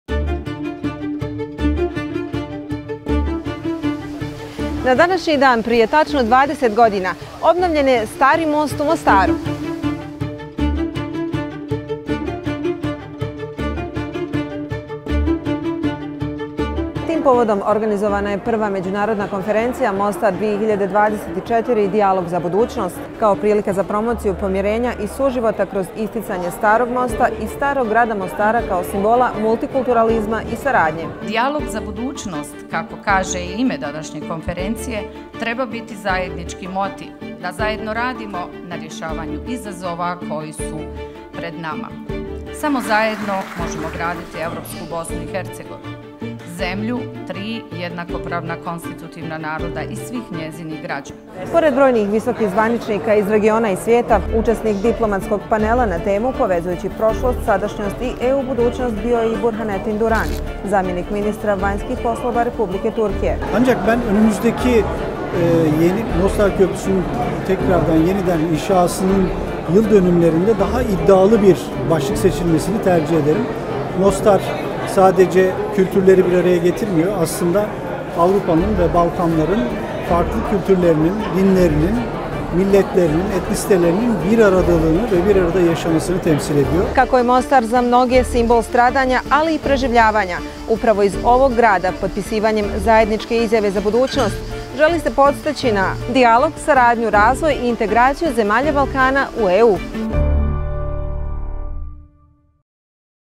Dijalog za budućnost
Prije 20 godina obnovljen je Stari most u Mostaru, koji je srušen 1993. godine.

Tim povodom u Mostaru je održana Međunarodna konferencija "Mostar 2024: Dijalog za budućnost", s porukom da je Bosna i Hercegovina most koji spaja različite civilizacije, kulture, narode i religije.